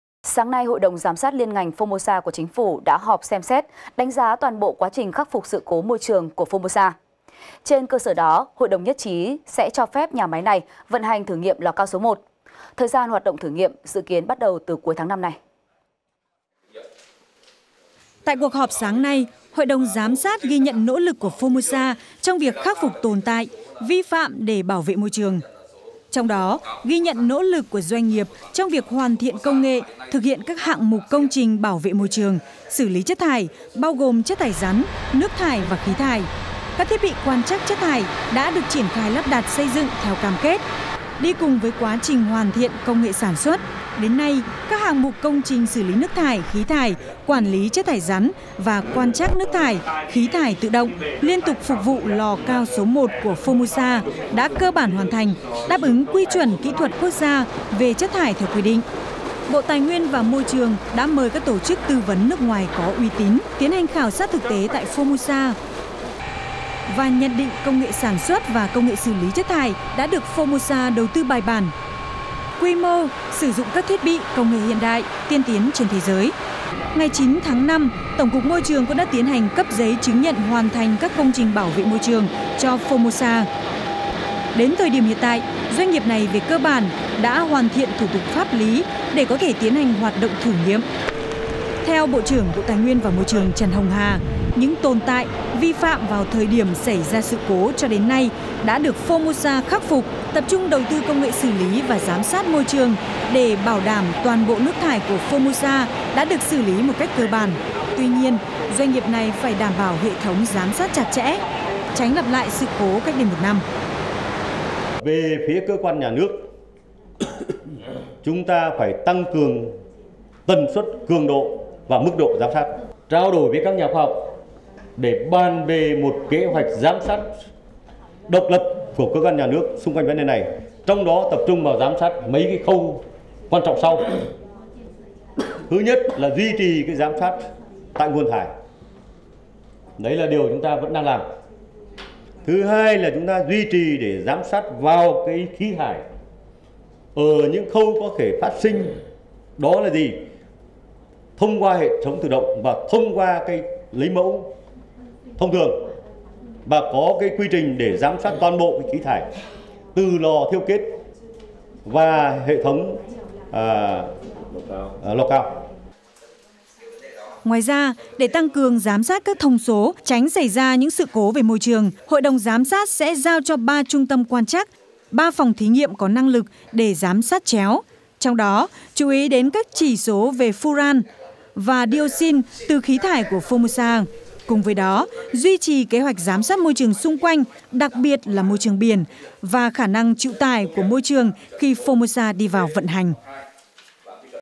(VTC14)_Cho phép Formosa vận hành thử nghiệm lò cao số 1 vào cuối tháng 5
(VTC14) - Sáng nay 10/05, Hội đồng giám sát liên ngành Formosa của Chính phủ đã họp xem xét, đánh giá toàn bộ quá trình khắc phục sự cố môi trường của Formosa. Trên cơ sở đó, Hội đồng nhất trí sẽ cho phép nhà máy này vận hành thử nghiệm lò cao số 1. Thời gian hoạt động thử nghiệm dự kiến sẽ bắt đầu từ cuối tháng 5 này.